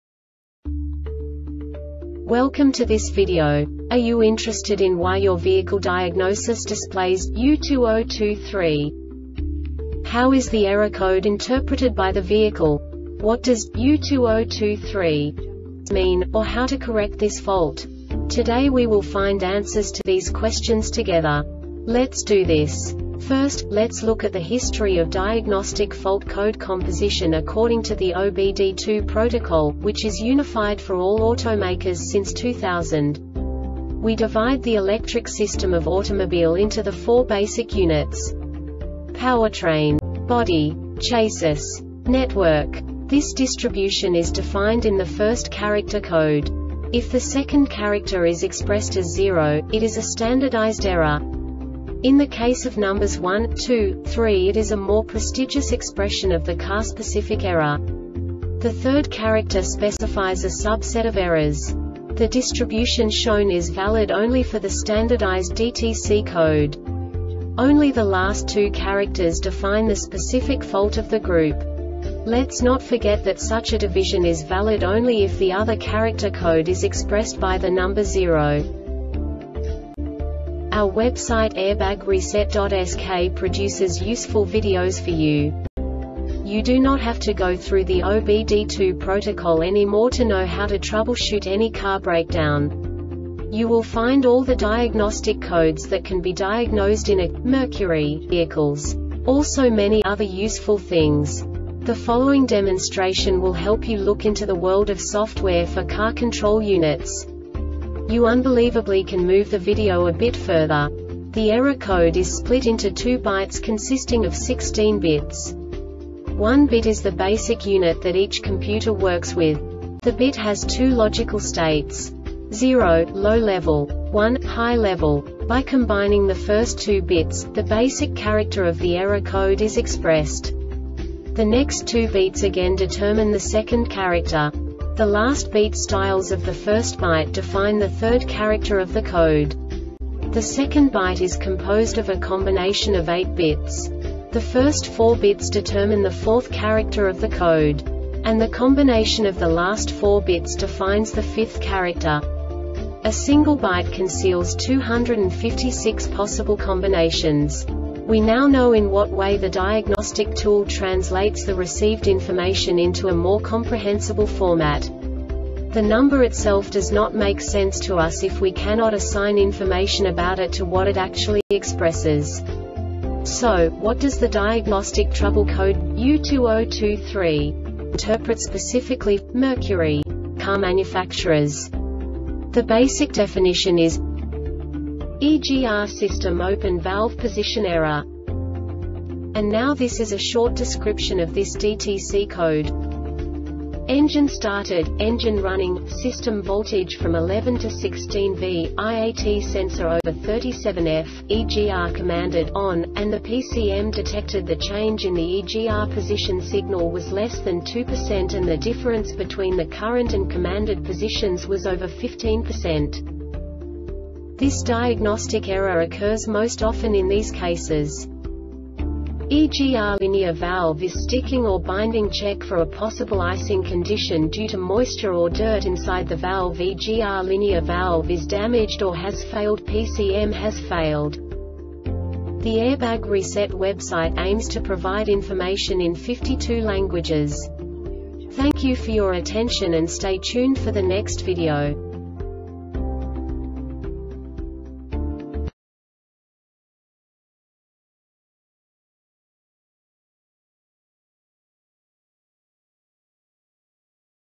DTC Mercury U2023 Short Explanation
Contents:
0:21 Basic DTC analysis according to OBD2 protocol standard.
1:48 Insight into programming
2:58 A diagnostically understandable description of the Diagnostic Trouble Code
3:05 Basic error definition

Make:
Mercury
Code:
U2023
Definition:
EGR System Open Valve Position Error
Description:
Engine started,engine running,system voltage from 11-16v,IAT sensor over 37ºF,EGR commanded "on",and the PCM detected the change in the EGR position signal was less than 2% and the difference between the current and commanded positions was over 15% w
Cause:
EGR Linear valve is sticking or binding (check for a possible icing condition due to moisture or dirt inside the valve)EGR Linear valve is damaged or has failedPCM has failed